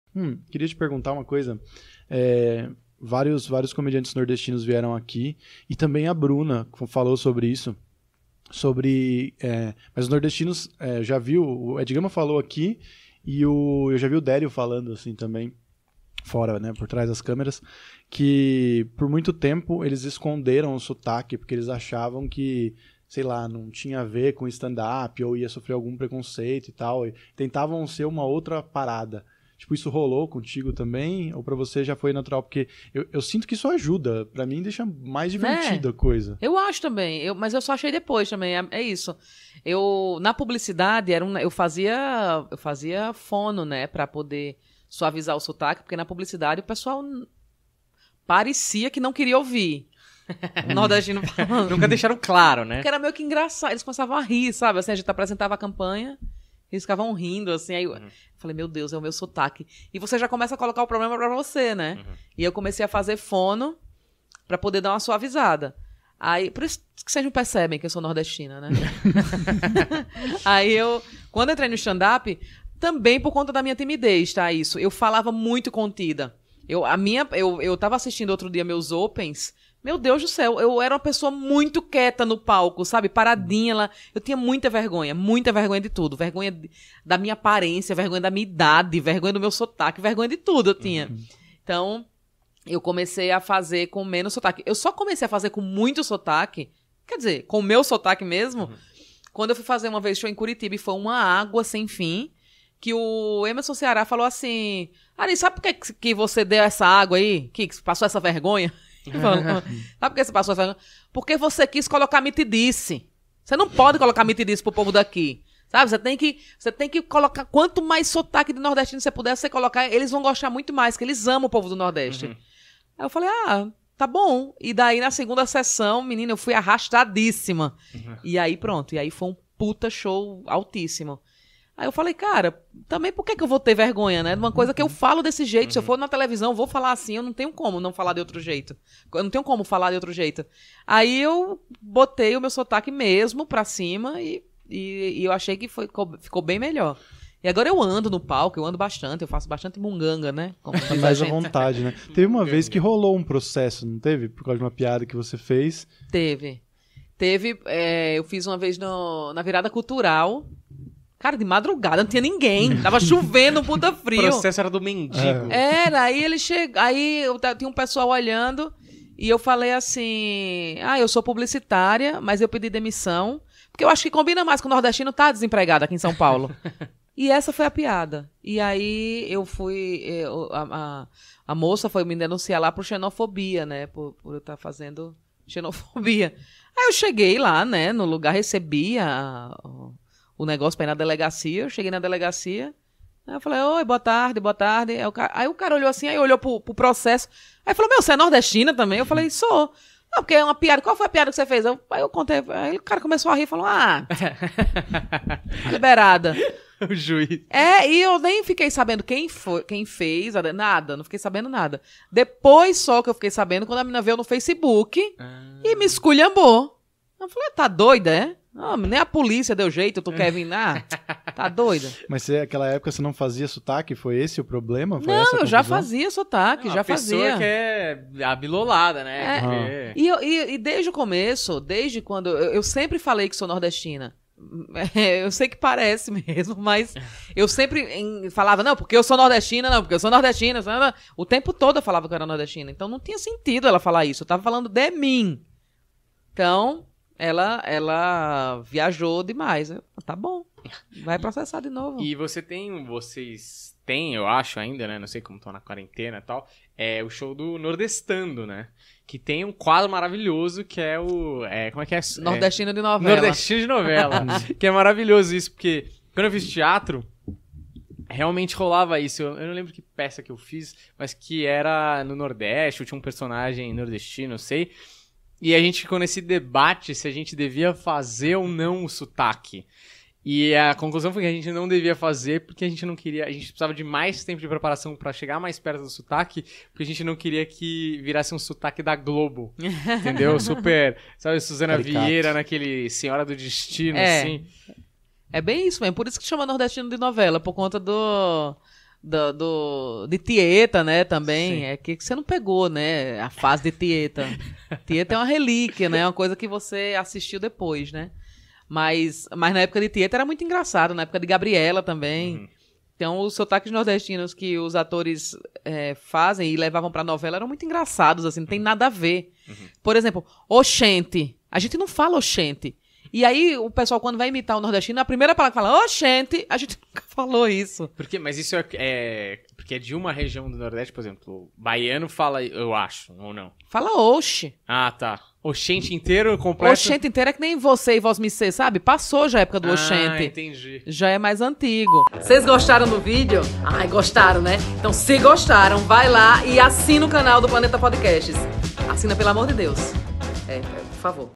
NORDESTINOS NO STAND UP com Arianna Nutt | Planeta Podcast
Arianna Nutt, integrante da Culpa é da Carlota, conta uma história em que foi processada por ser xenofóbica, por conta de piadas que contava no palco sobre ser nordestina, algo bem estranho já que Arianna é do nordeste!